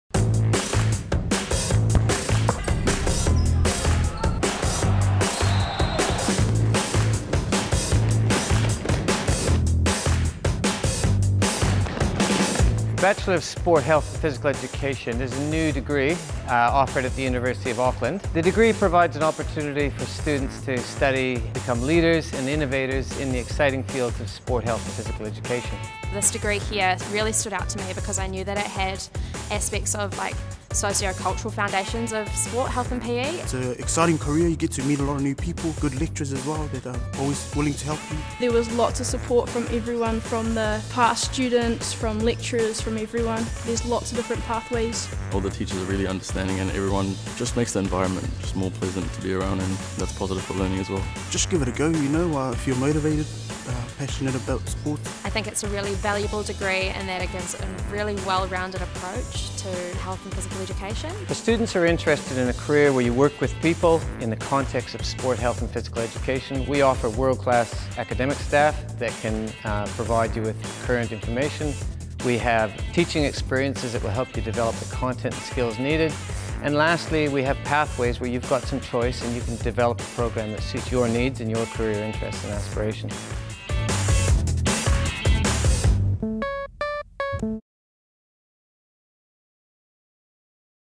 Bachelor of Sport, Health and Physical Education
Become a leader and innovator in the exciting fields of sport, health and physical education with the BSportHPE degree at the University of Auckland.